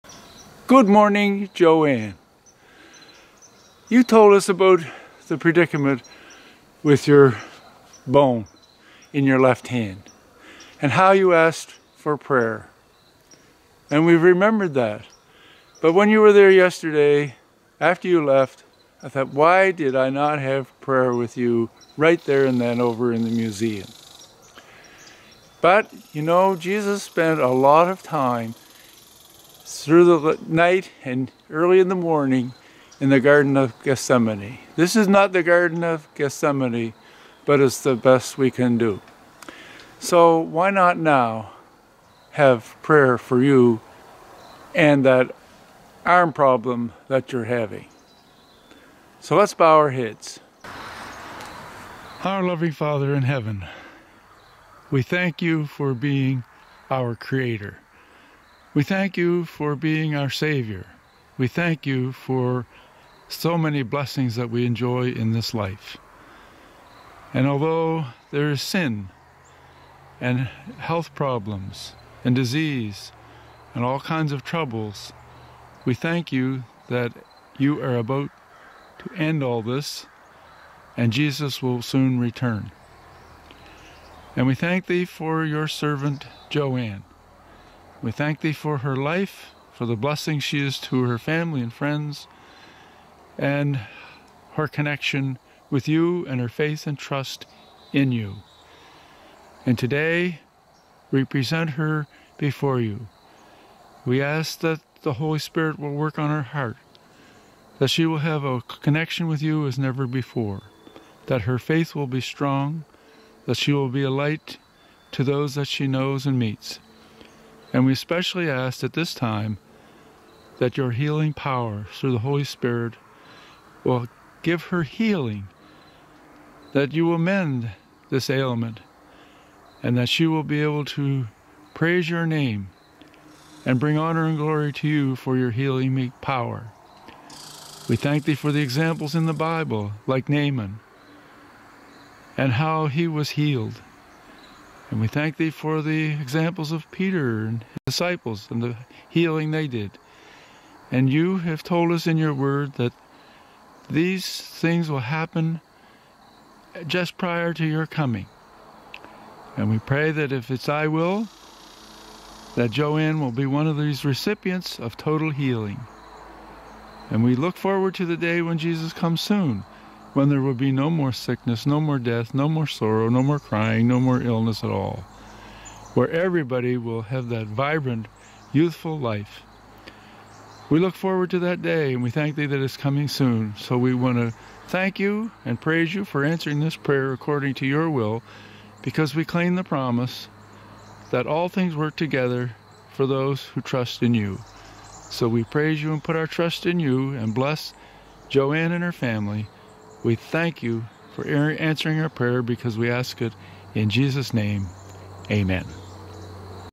For Jo Anne
Jo Anne asked us to have prayer that the Lord will heal her bones. It's a privilege for us to participate in her request.